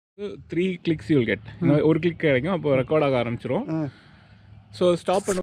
Walk Around Type R and Triumph 660 triple
Walk around outside Sharath s house and his truimph bike